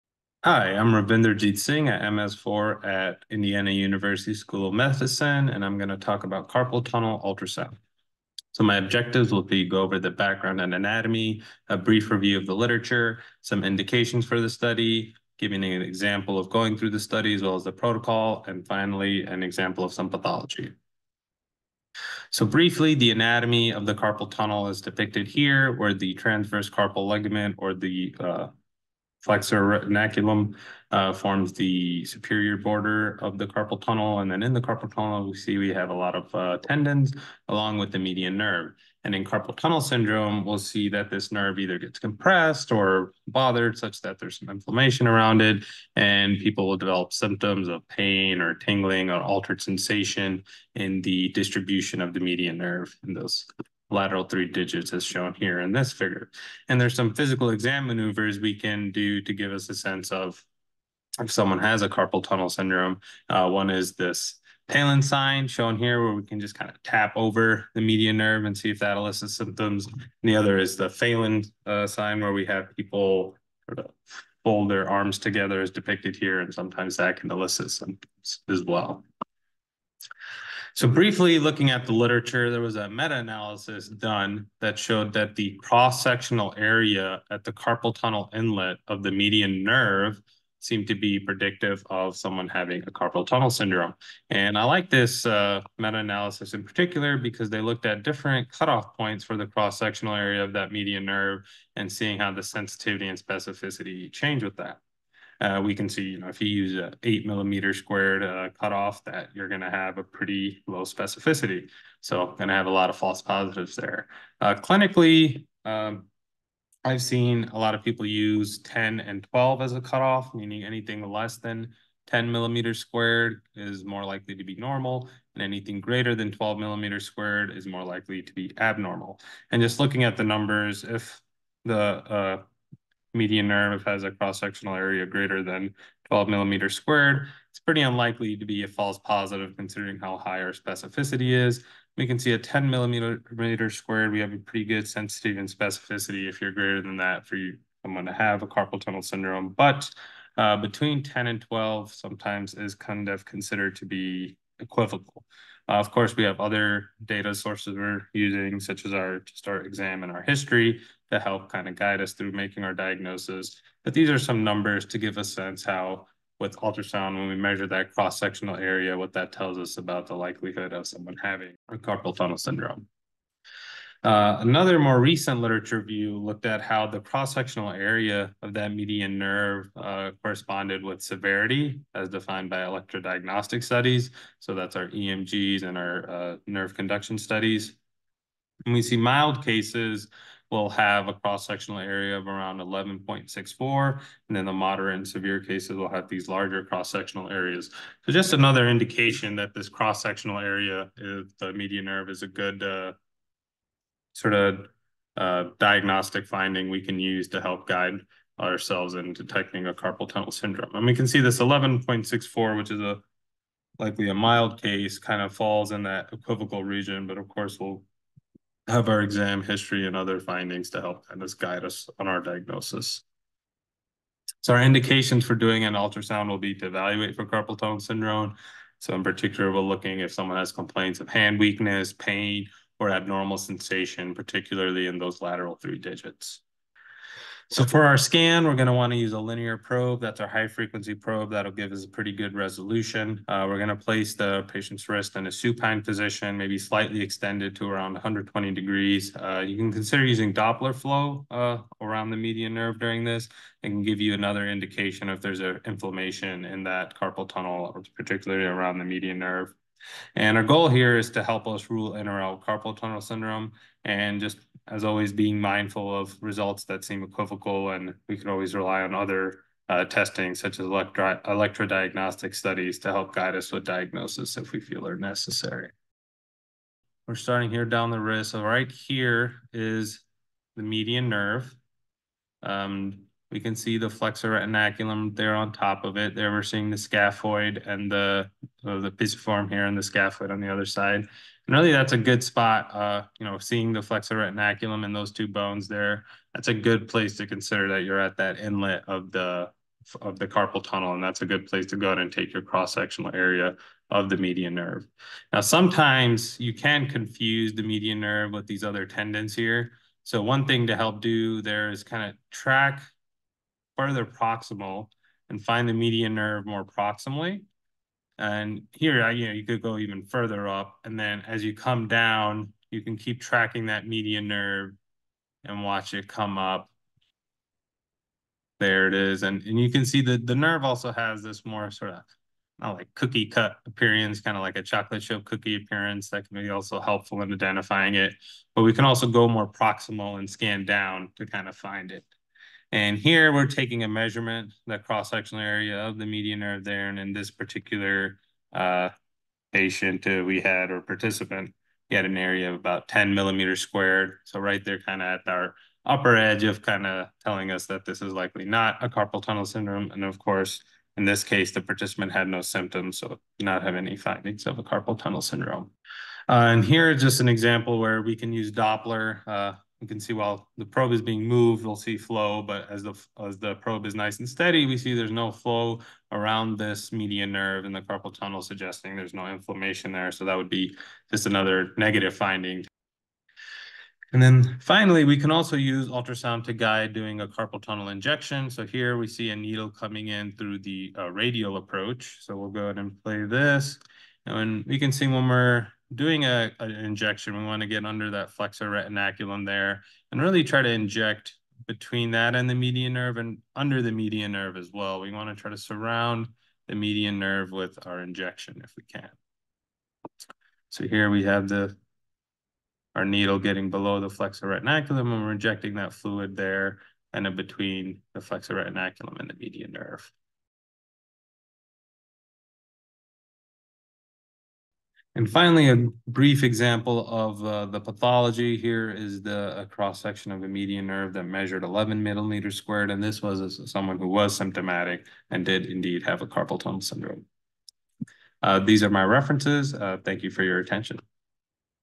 Carpal Tunnel Syndrome
Ravinderjiit walks us through how to evaluate carpal tunnel syndrome with ultrasound.